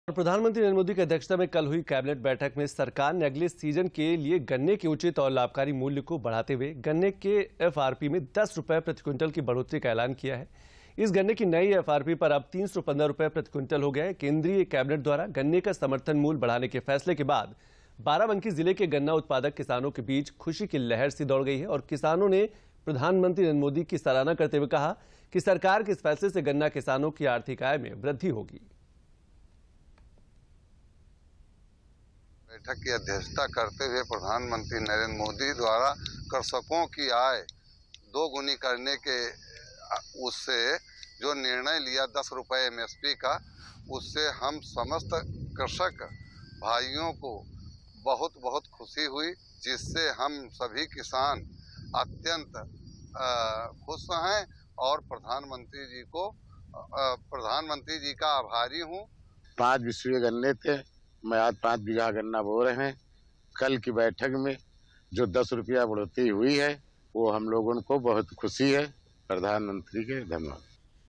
Barabanki जिले के गन्ना उत्पादक किसानों के बीच खुशी की लहर
प्रधानमंत्री नरेंद्र मोदी की अध्यक्षता में कल हुई कैबिनेट बैठक में सरकार ने अगले सीजन के लिए गन्ने के उचित और लाभकारी मूल्य को बढ़ाते हुए गन्ने के एफआरपी में 10 रुपये प्रति क्विंटल की बढ़ोतरी का ऐलान किया है। इससे गन्ने की नई एफआरपी अब 315 रुपये प्रति क्विंटल हो गया है। केंद्रीय कैबिनेट द्वारा गन्ने का समर्थन मूल्य बढ़ाने के फैसले के बाद बाराबंकी जिले के गन्ना उत्पादक किसानों के बीच खुशी की लहर सी दौड़ गई है और किसानों ने प्रधानमंत्री नरेन्द्र मोदी की सराहना करते कहा कि सरकार के इस फैसले से गन्ना किसानों की आर्थिक आय में वृद्धि होगी।